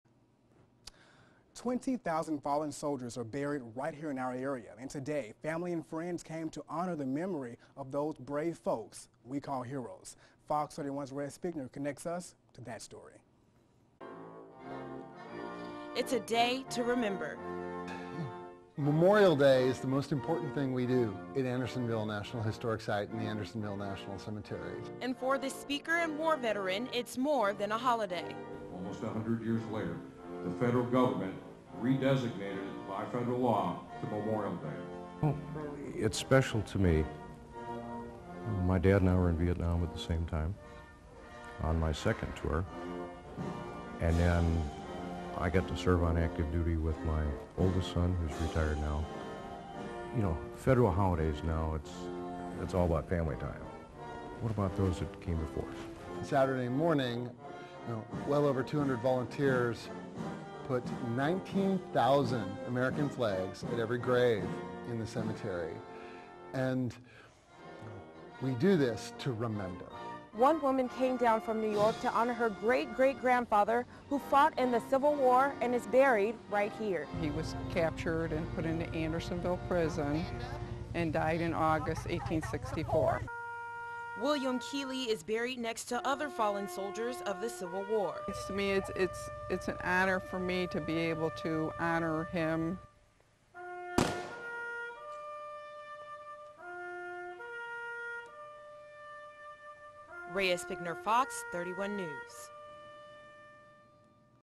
Soldiers and veterans honored Sunday
Andersonville Historic Site and National Cemetery held a memorial service for veterans and fallen soliders Sunday afternoon.FOX 31 welcomes and appreciates comments from all viewers; however, any and all comments that we deem inappropriate or offensive will be moderated.